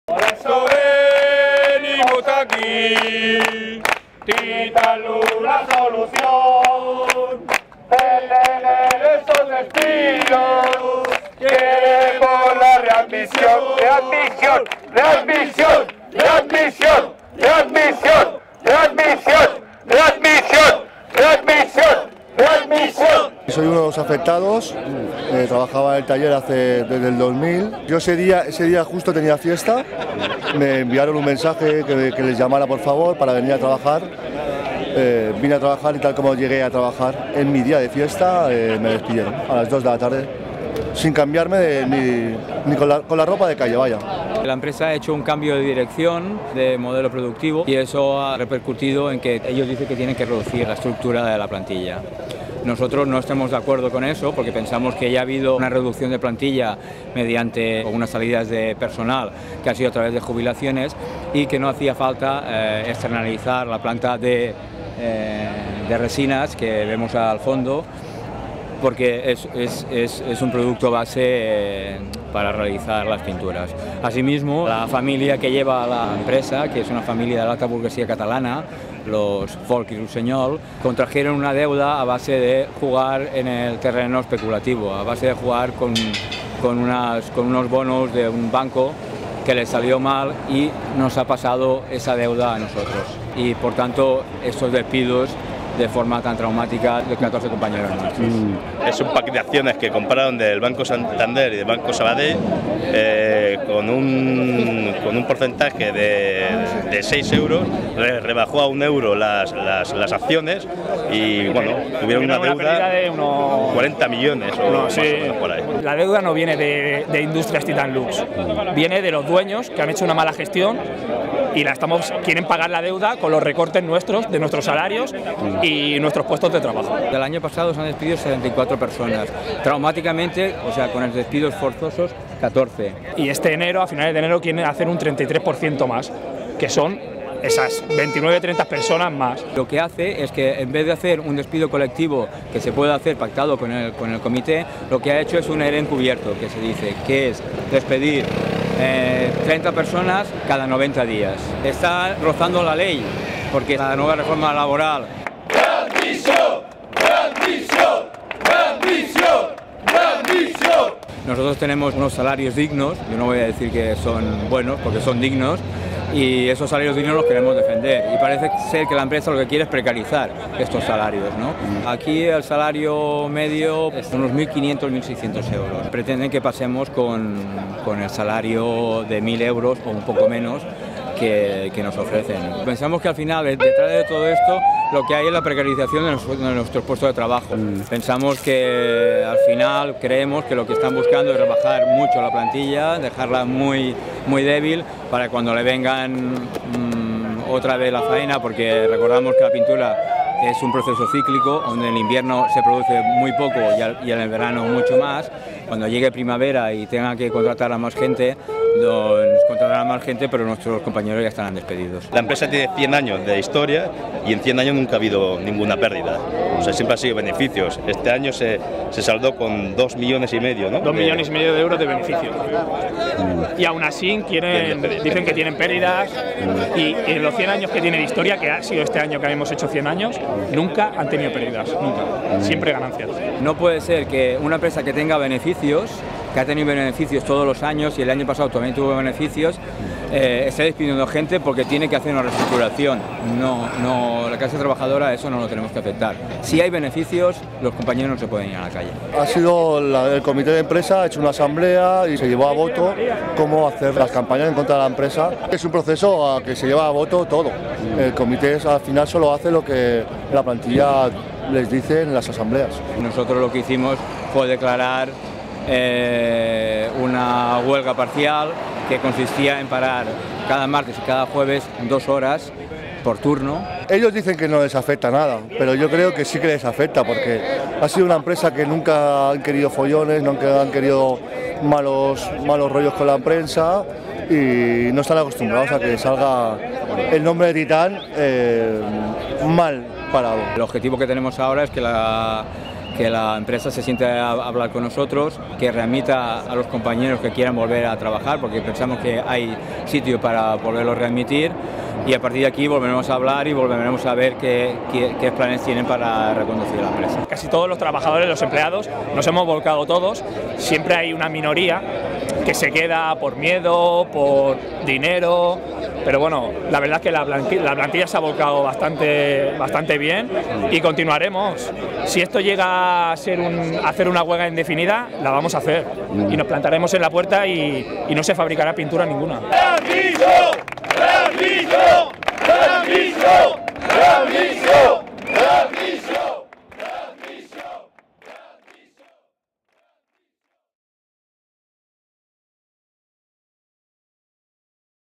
Barcelona: Titanlux strike/Huelga en Titanlux
***DESCRIPCIÓN EN INGLES, Y DESPUES EN CASTELLANO***
Workers at the Titanlux paint factory in Barcelona are on strike. 14 workers have been laid off with another 30 due to be sacked on January 27, and the remaining workers are expected to take a pay cut from around €1500 - €1600 a month to €1000 a month. Although the boses say it's a restructure, in reality it's a way of paying off a €40 million debt they have from gambling on the stock market. The workers are determined not to pay for their bad management, and are prepared to go on all-out indefinite strike if necessary.
Los trabajadores de la fábrica de pinturas de Titanlux en Barcelona están en huelga. 14 trabajadores han sido despedidos, 30 mas va a ser despedidos el 27 de enero, y se espera que los trabajadores restantes tomen un recorte salarial de alrededor de € 1500 - € 1600 por mes a € 1000 por mes. Aunque los jefes dicen que es una reestructuración, en realidad es una forma de pagar una deuda de € 40 millones que tienen por apostar en el mercado de valores. Los trabajadores están decididos a no pagar por su mala gestión y están preparados para realizar una huelga indefinida si es necesario.